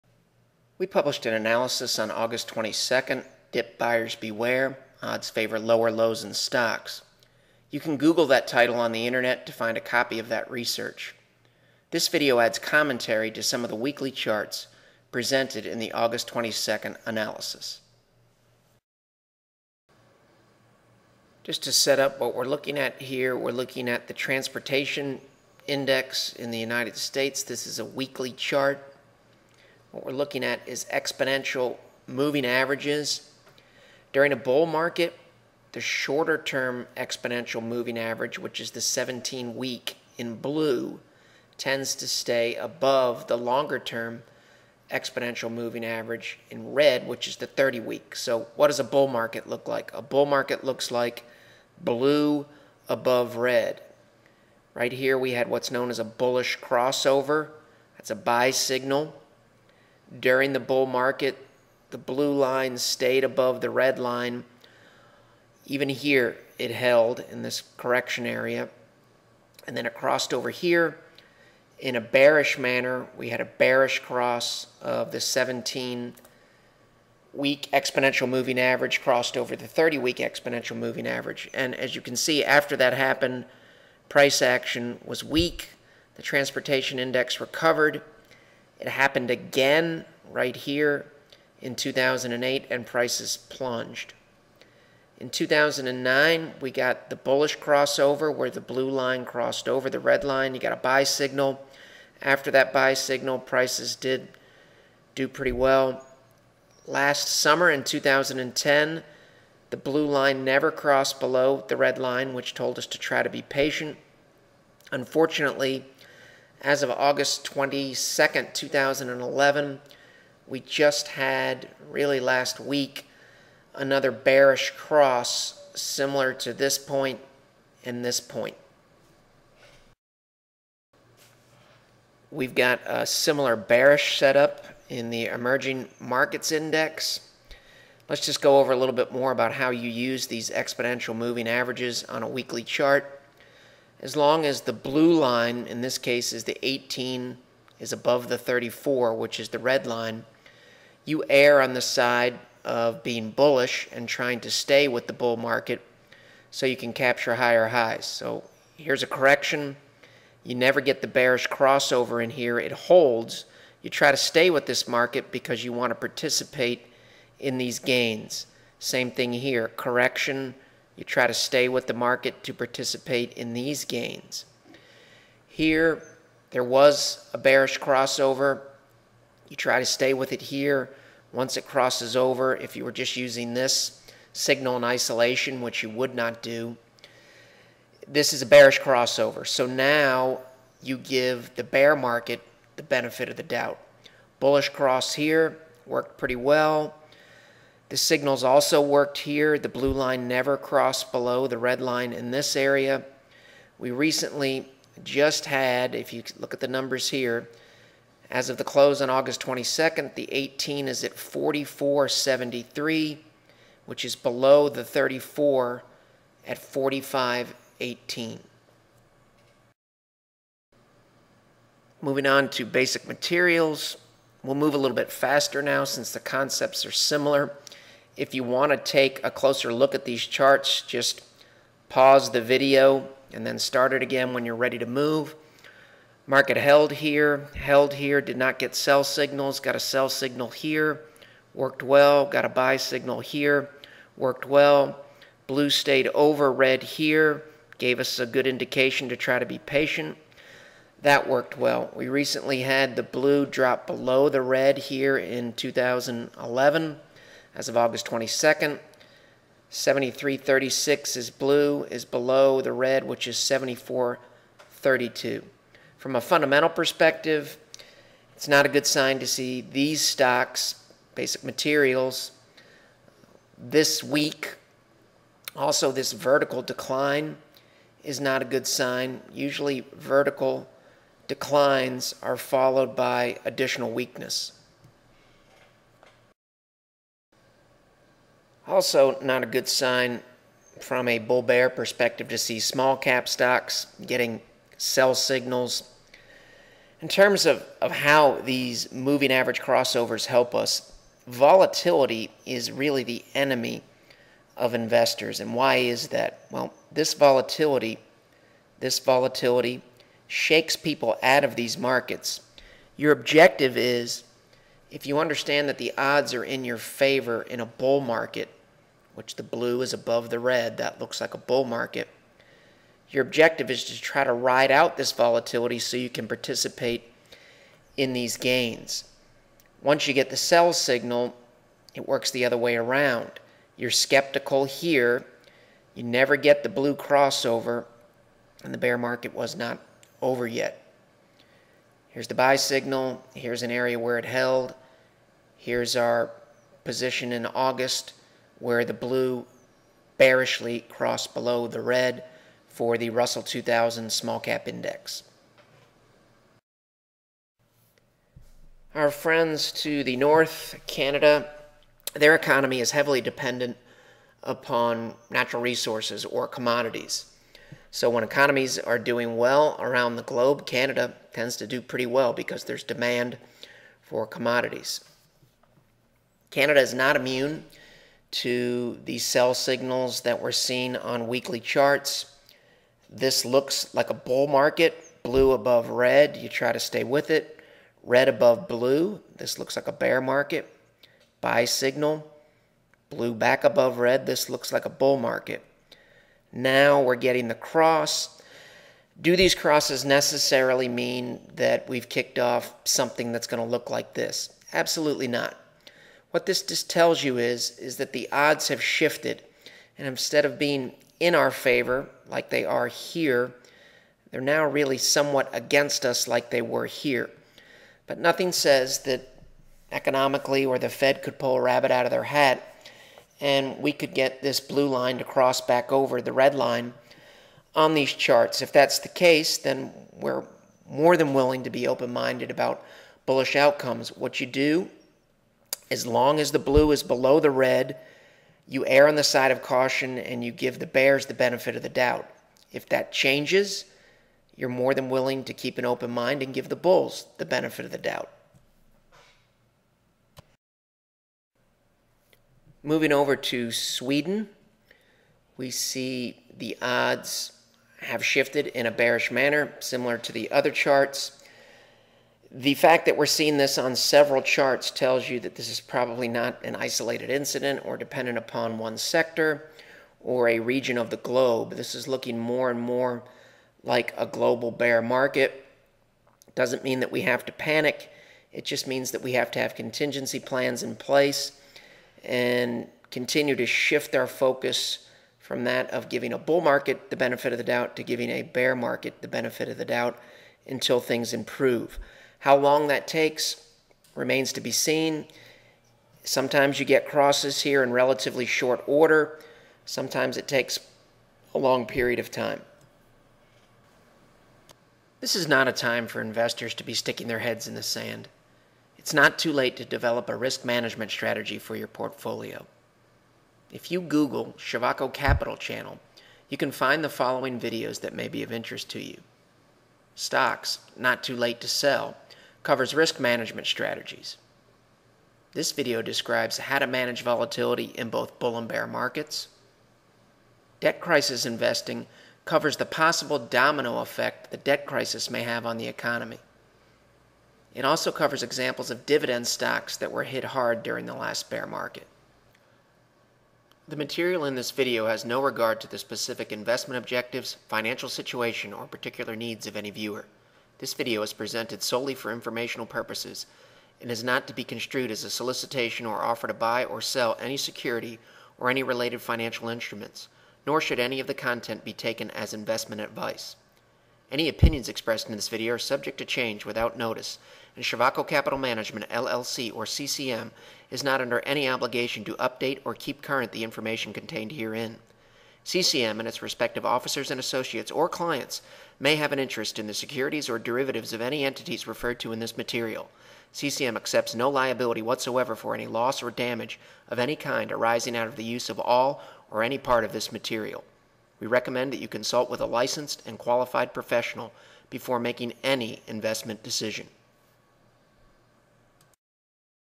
Bearish Signals Global Stocks
Long-Term bearish signals are appearing on many weekly stock charts.  Money Manager Chris Ciovacco reviews bearish developments on the charts of transportation stocks (IYT), emerging markets (EEM), basic materials (IYM), small caps (IWM), Canada (EWC), and Sweden (EWD).

Bear market investing strategies are discussed, as well as what to look for in terms of bullish outcomes for stocks and commodities.  Technical analysis hints at a new bear market in terms of probabilities.